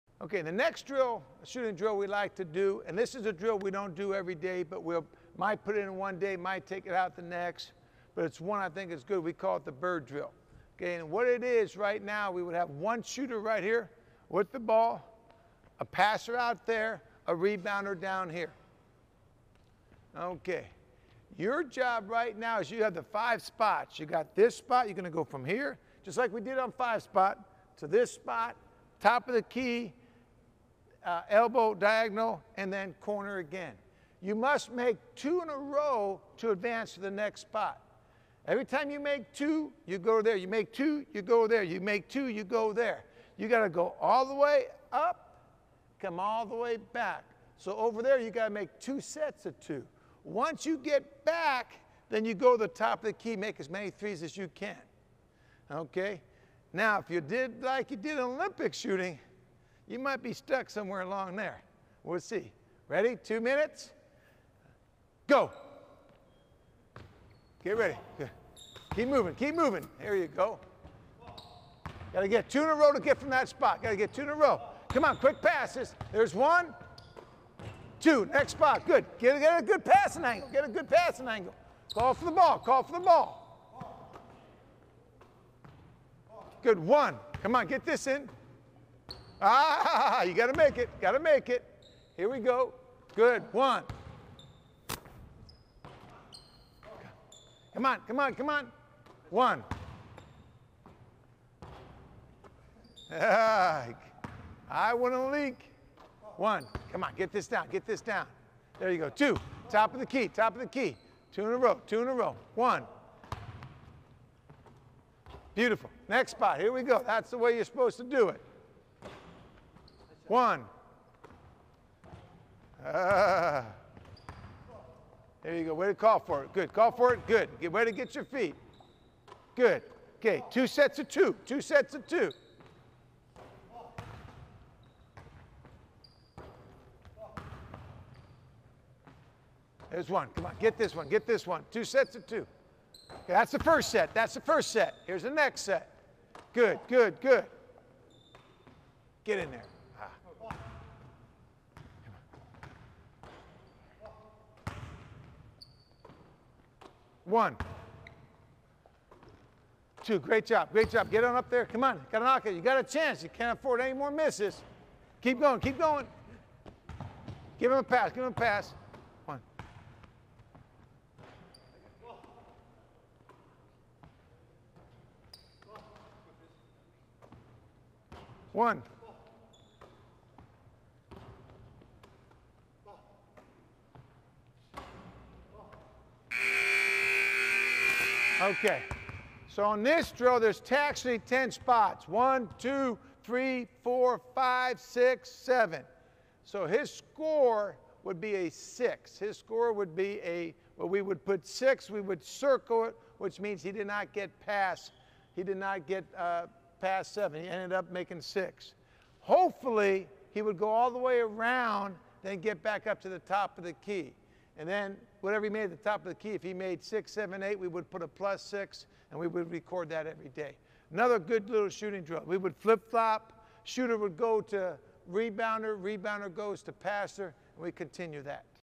Master the Innovative Dribble Drive Attack Offense!
Creator of the innovative Dribble Drive Attack Offense, Vance Walberg, tells you his philosophy of  "3 or Key", meaning he wants the ball driven into the key and if the defense collapses then the ball should be kicked out for a 3-point shot. Walberg likes the 3-point shot because it can yield a higher Points Per Possession average than the mid-range jumpers.